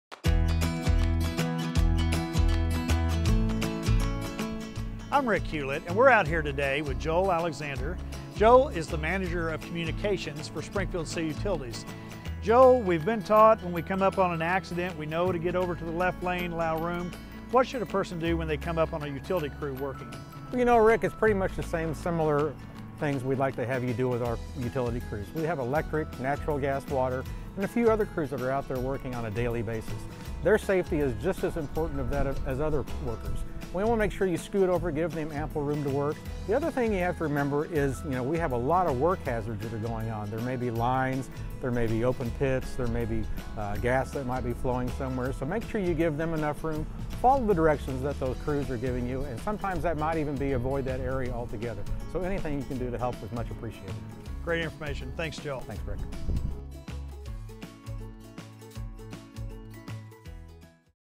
Rick's Automotive – Utility Work Zone
When coming up on a utility work zone, be sure to give the crew plenty of room to work. Their safety is just as important as other workers. Work hazards can be nearby, so be sure to follow the directions you are given.